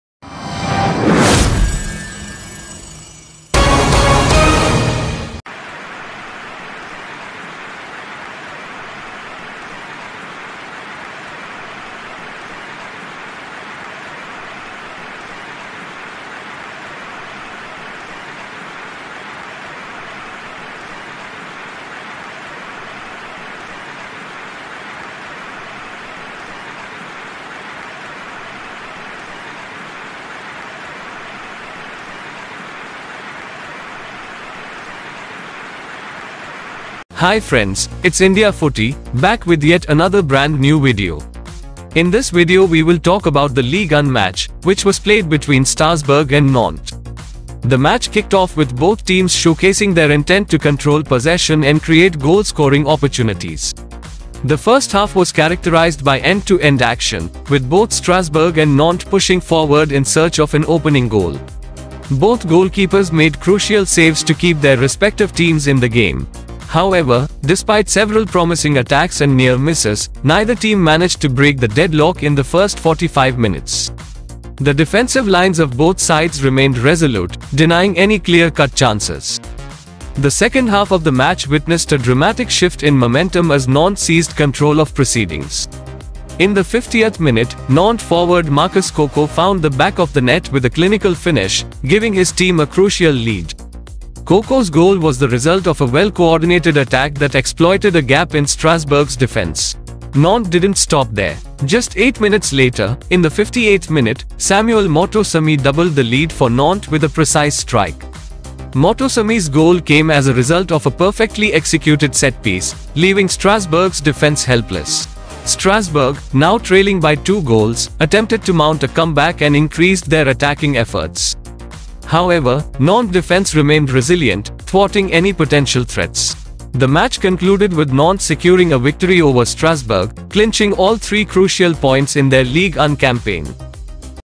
Strasbourg Nantes | 1-2 | Le résumé | Ligue 1 2023/24
Le match entre Strasbourg et Nantes a offert aux supporters une démonstration passionnante du football en Ligue 1. Nantes est sorti vainqueur avec une victoire 2-1, grâce aux buts de Marcus Coco et Samuel Moutoussamy. Si Strasbourg a vaillamment riposté et a réussi à marquer un but grâce à Moise Sahi, cela n'a pas suffi pour assurer un retour.